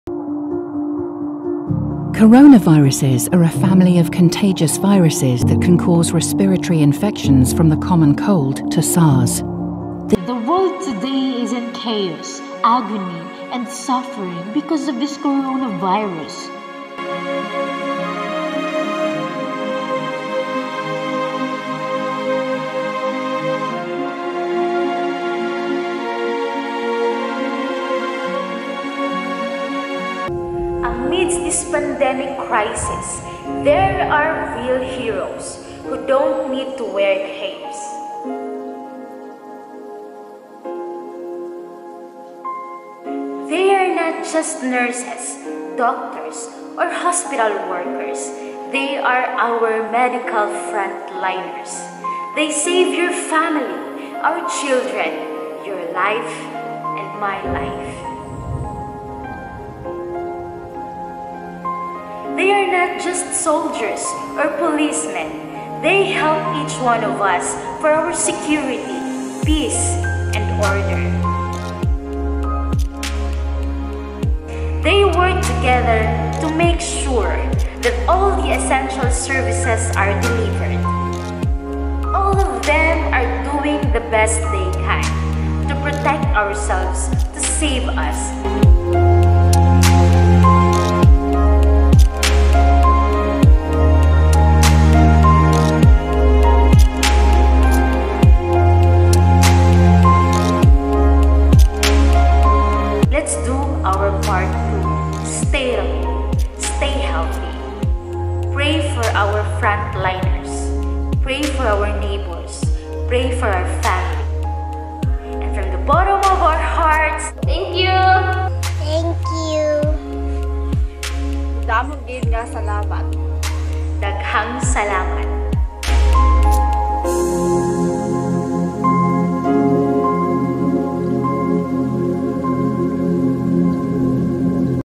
COVID 19 Frontliners - THANK YOU
Hey there amazing people, sharing this short video vlog as a way of appreciation to our frontliners -our modern heroes today. They're doing their best to help and save the humanity against this pandemic Corona Virus.